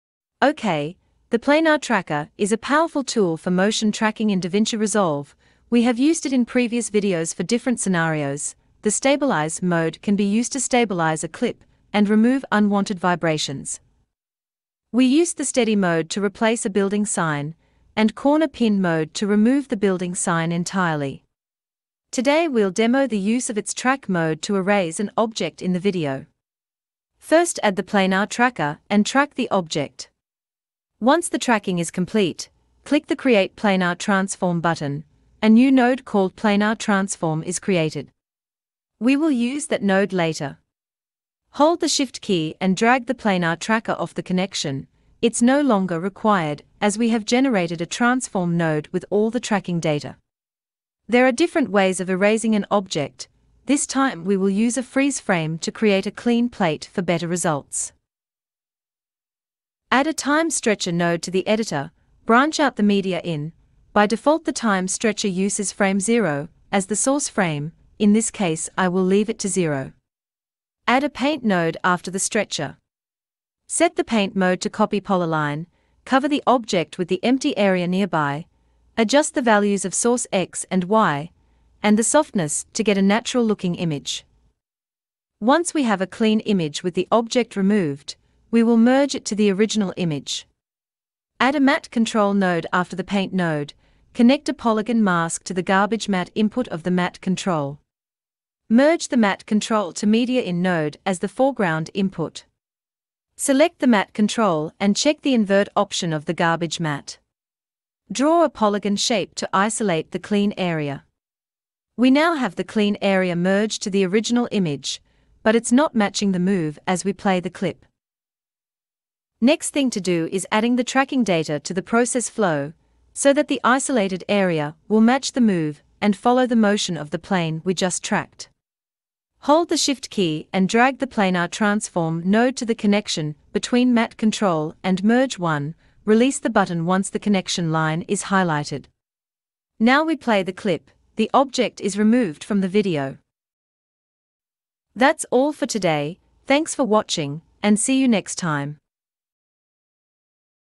Motion Tracking - Erase Object using Fusion Paint Tool and Planar Tracker in DaVinci Resolve
In this video, we use paint tool to erase an object from the video and match move the clip with track mode of the Planar Tracker.